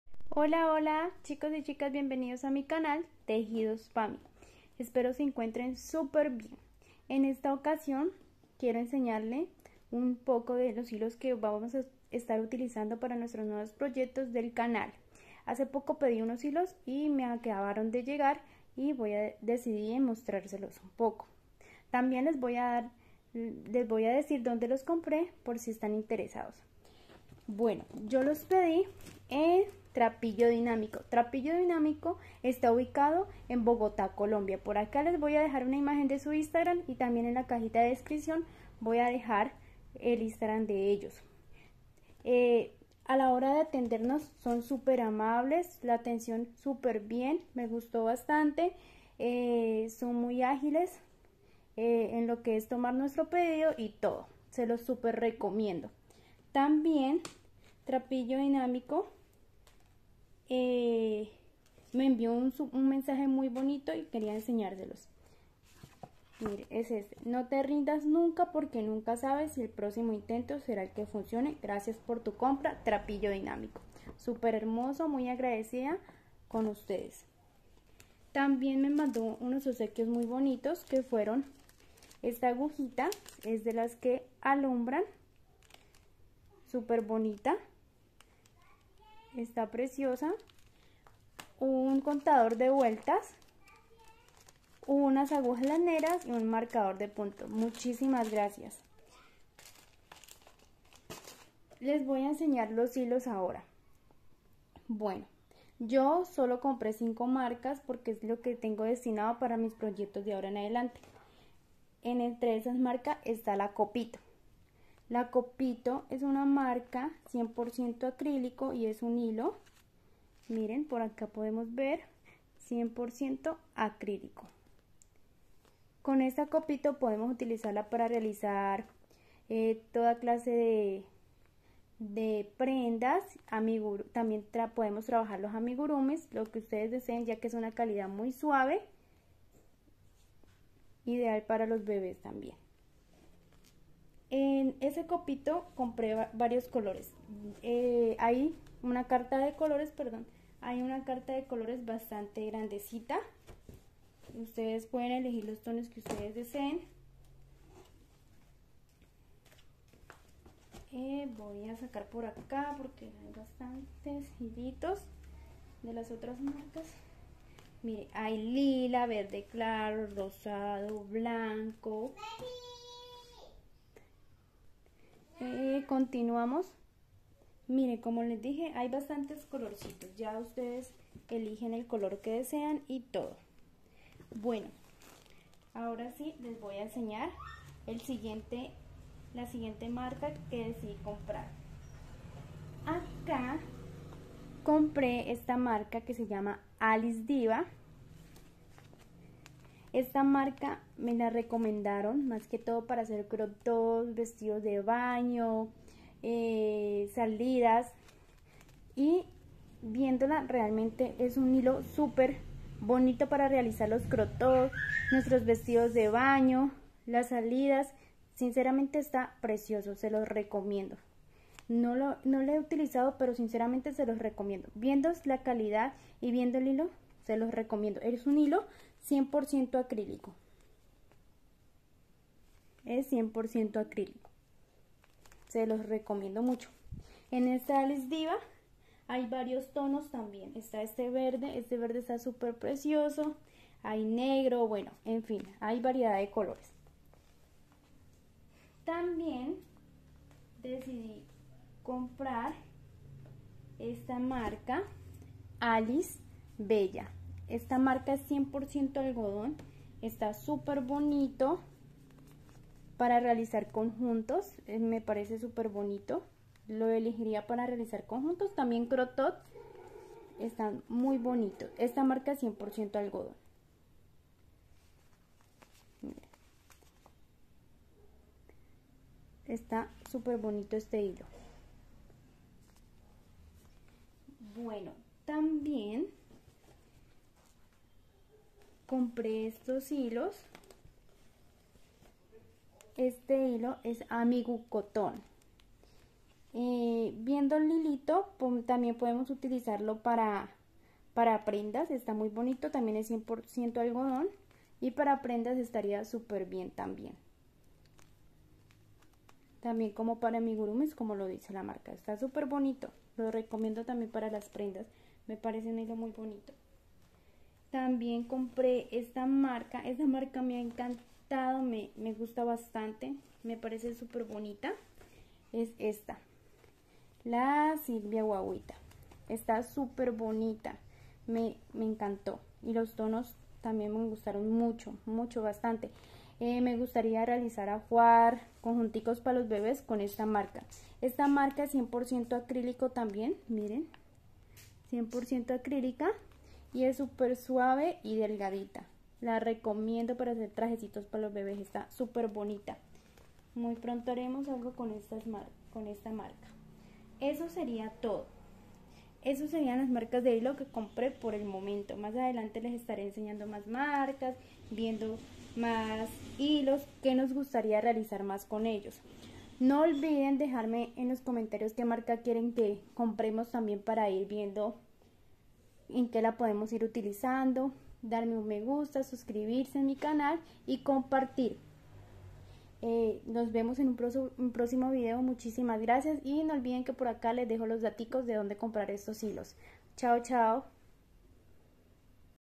5 Clases de hilos acrílico y algodón
Hola 👋
BIENVENIDOS A MI CANAL TEJIDOS PAMI 🧶
En esta oportunidad les muestro  5 marcas de hilos entre acrílicos y algodón 🧶, que estaremos trabajando en nuestro canal 💖

Se las recomiendo 🧶